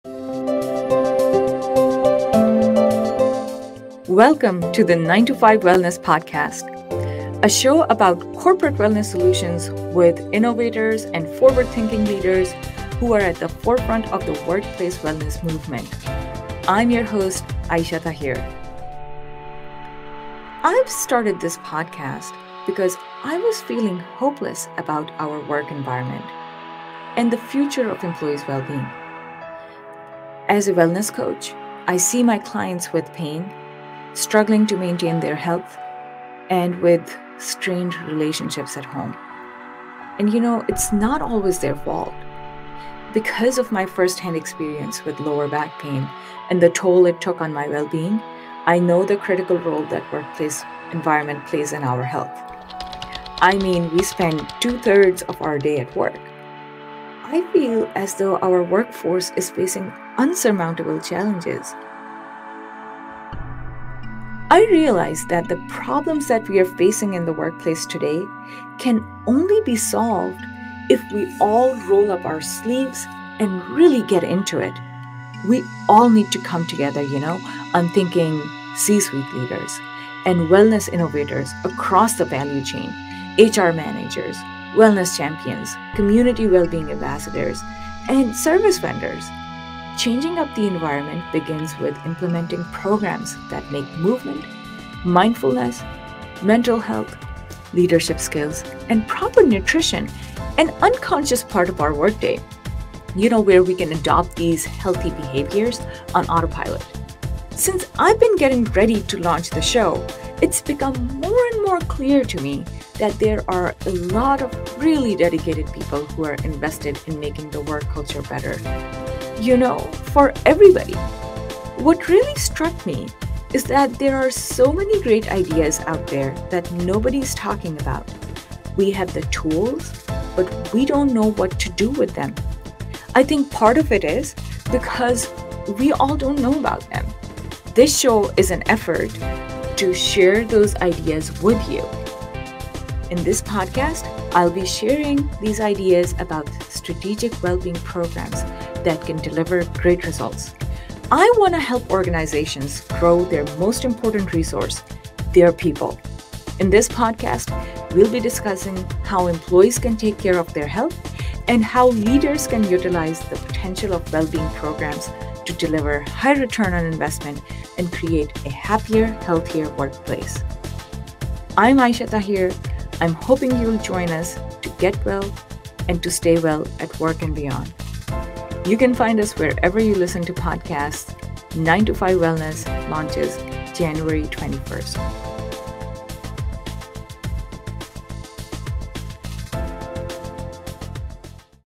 9 to 5 Wellness Podcast Trailer
9 to 5 Wellness Podcast is a show about corporate wellness solutions with innovators and forward-thinking leaders who are at the forefront of the workplace wellness movement.

Curious what it’s about?

On this show, I'll be diving into meaningful discussions with industry leaders from healthcare, technology, finance, human resources, pharmaceuticals, government, and more. We'll explore insights on investing in human capital, such as:

🌟 What has helped their organizations gain a competitive advantage?

🌟 How do they envision the future of employee wellbeing?

🌟 Addressing misunderstandings prevalent in the corporate market.

🌟 Providing advice to other leaders for creating a happier & healthier workplace.

🎧🎧🎧 9 to 5 Wellness is my contribution to the wellness industry, busy office workers, and C-suite leadership

🎧 It is in service to the hardworking employees and leaders of every organization who I aim to bring wellness to through meaningful discussions with industry leaders.